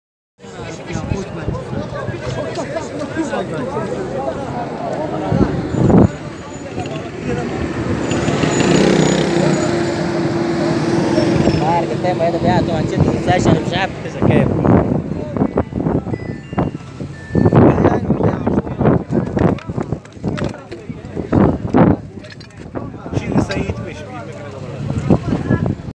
Arresting Afghan nationals in Iran's Baluchistan
An Afghan man was caught in Iran's Balochistan by the Iranian police